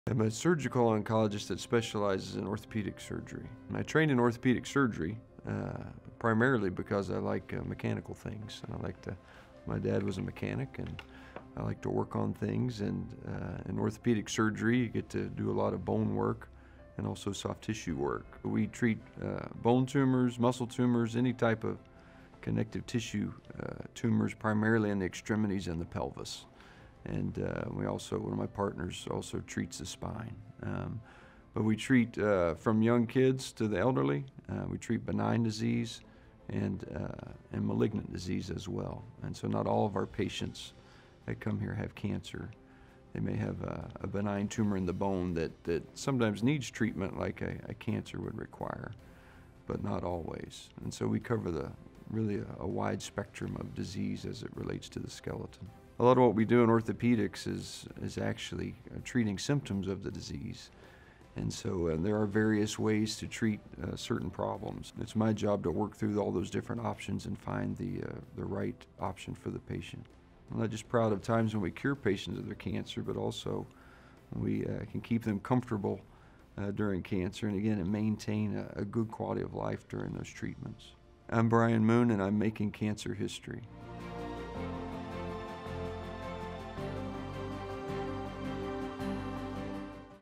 Meet orthopaedic oncologist Bryan Moon, M.D.
Bryan Moon, M.D., is a professor of Orthopaedic Oncology at The University of Texas MD Anderson Cancer Center in Houston, Texas. He joined MD Anderson in December 2008 as a clinical, non-tenure track faculty member. Dr. Moon has extensive experience in the treatment of benign and malignant musculoskeletal neoplasms with a special interest in metastatic disease and limb salvage. In addition, he is the associate director of the Musculoskeletal Oncology Fellowship program and the clinical dyad leader for the Care Planning Operational Priority. Dr. Moon has been rated in the top 1% nationally on CAHPS patient care measures. He has published multiple manuscripts and authored several book chapters.